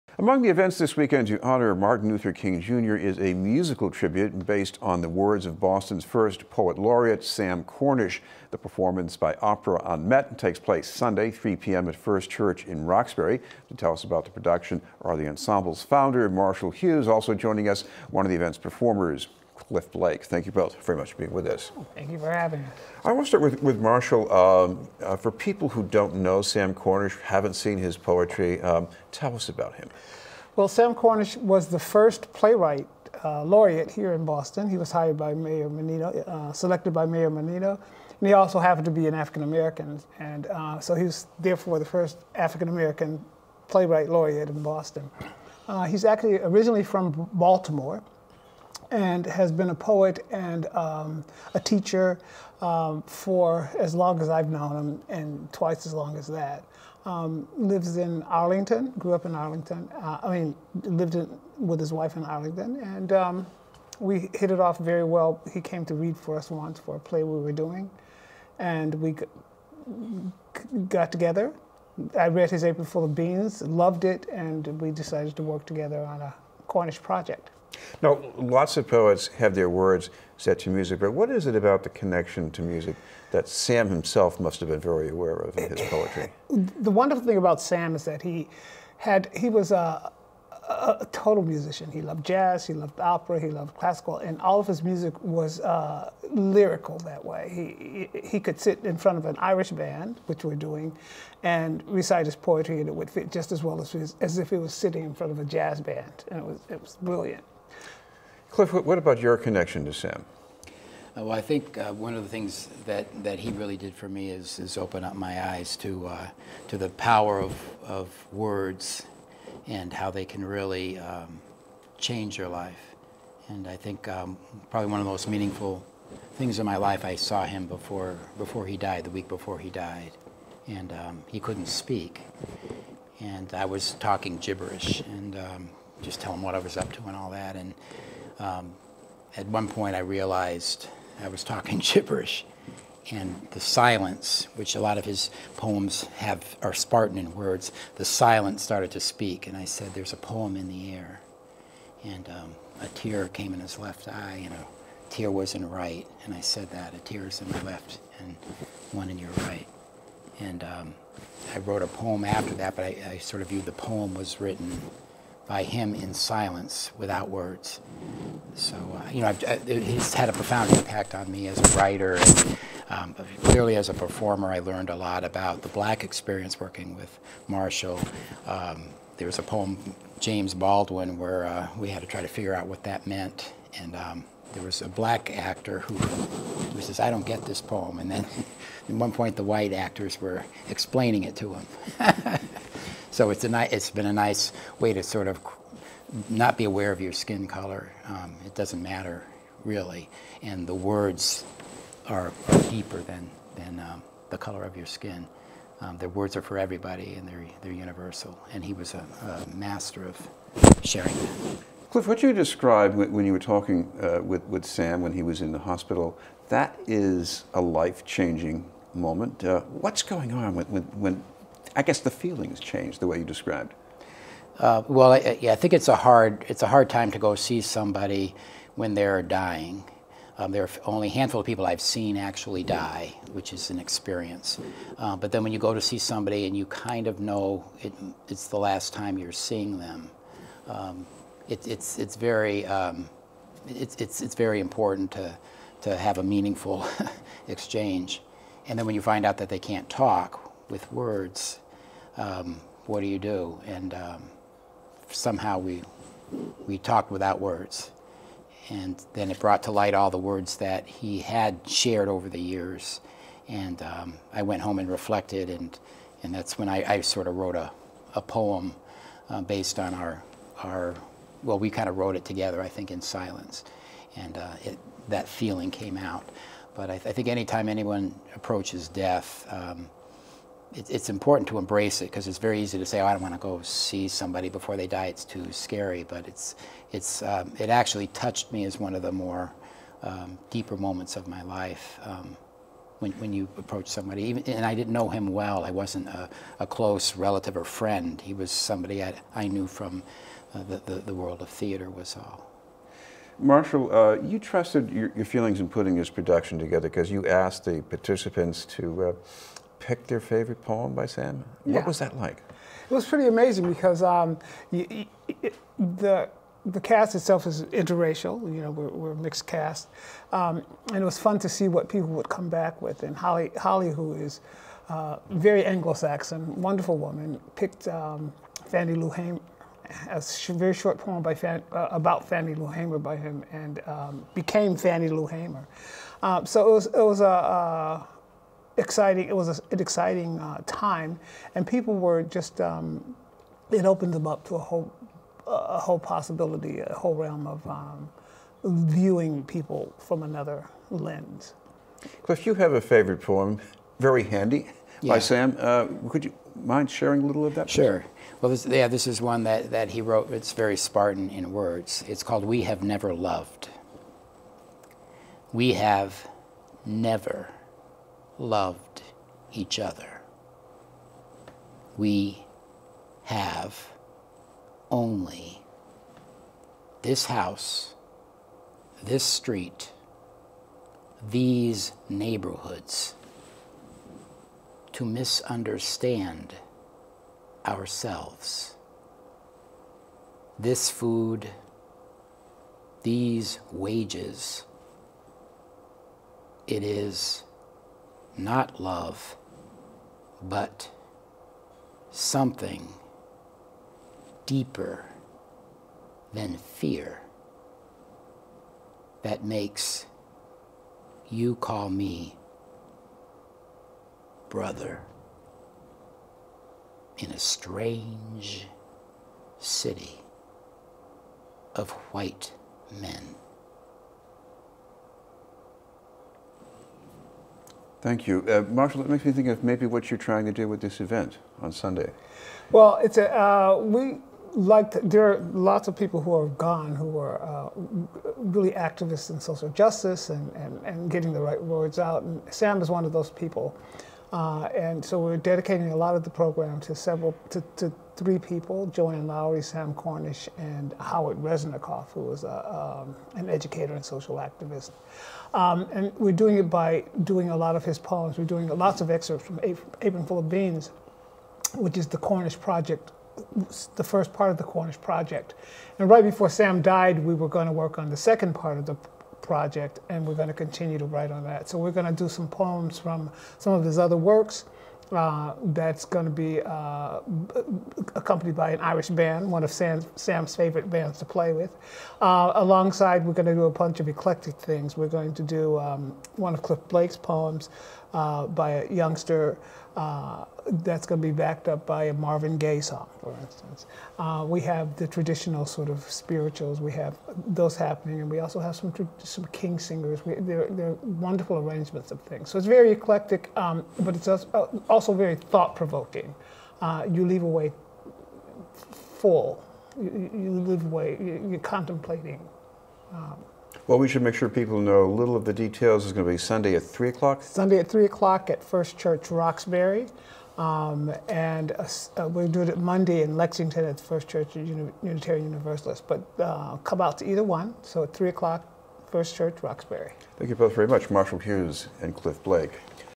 "Ties That Bind" to Honor MLK
Marshall Hughes of Opera unMet and performer/poet Cliff Blake talk about the January 20 presentation of "The Ties That Bind," a tribute to MLK featuring the words of Boston's first Poet Laureate, Sam Cornish. Interview for BNN News. Aired January 16, 2019.